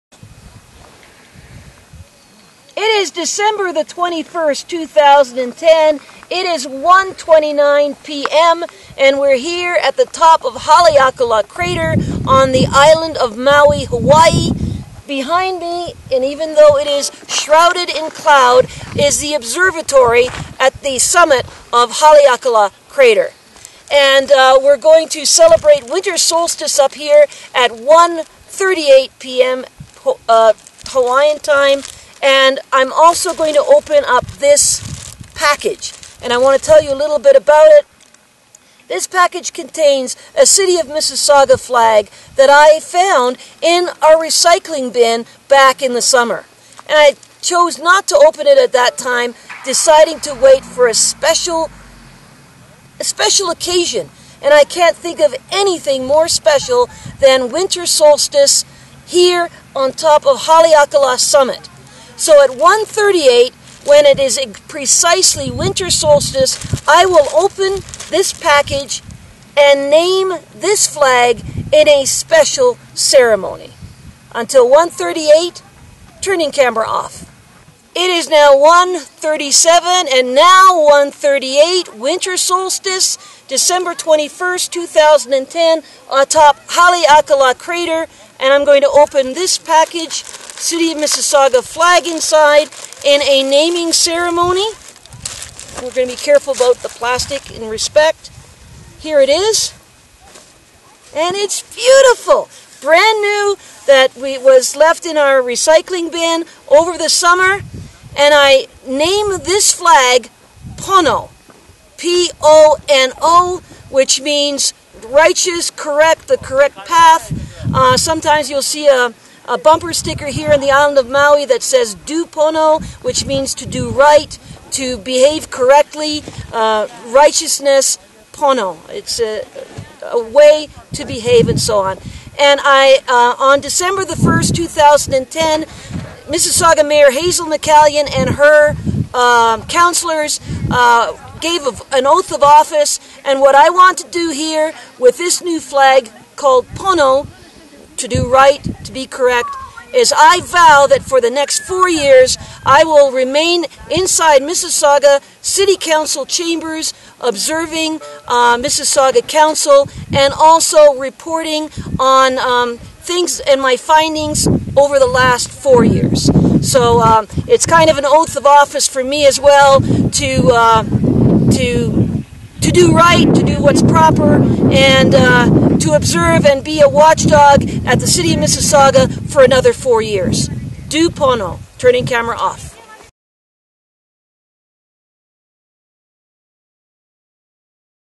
Winter Solstice, Haleakala crater, MISSISSAUGAWATCH Oath of "Office" ceremony. December 21, 2010
VIDEO TRANSCRIPT (December 21, 2010 1:38 Hawaiian time)

MISSISSAUGAWATCH (Winter Solstice, December 21, 2010 atop summit Haleakala, Maui, Hawaii):

It is December 21st, 2010. It is 1:29 pm and we're here atop Haleakala crater on the island of Maui, Hawaii. Behind me and even though it is shrouded in cloud is the observatory at the summit of Haleakala crater.

And we're going to celebrate Winter Solstice up here at 1:38 pm Hawaiian time.

And I'm also going to open up this package. And  I want to tell you a little about it.

This package contains a City of Mississauga flag that I found in our recycling bin back in the summer.  And I chose not to open it at that time deciding a special occasion. And I can't think of anything more special that Winter Solstice here on top of Haleakala summit.

So at 1:38, when it is precisely Winter Solstice, I will open this package and name this flag in a special ceremony.

Until 1:38, turning camera off.

[DIP TO WHITE]

It is 1:37 and now 1:38 —Winter Solstice, December 21st, 2010, atop Haleakala crater and I'm going to open this package —City of Mississauga flag inside, in a naming ceremony.We're going to be careful about the plastic in respect.

And it's beautiful! Brand new, that was left in our recycling bin over the summer. And I name this flag, "Pono" —P O N O, which means "righteous", "correct", "the correct path".

Sometimes you'll see a bumper sticker here on the island of Maui that says, "DO PONO", which means to do right, to behave correctly. Righteous. Pono. It's a way to behave and so on.

And on December 21st, 2010, Mississauga Mayor Hazel McCallion and her councillors gave an Oath of Office and what I want to do here with this new flag called "Pono" —to do right, to be correct— is—

I vow that for the next four years I will remain inside Mississauga City Council Chambers observing Mississauga Council and also reporting on things and my findings over the last four years.

So it's kind of an oath of office for me as well —to do right, to do what's proper and to observe and be a watchdog at the City of Mississauga for another four years.

"Do Pono".

Turning camera off.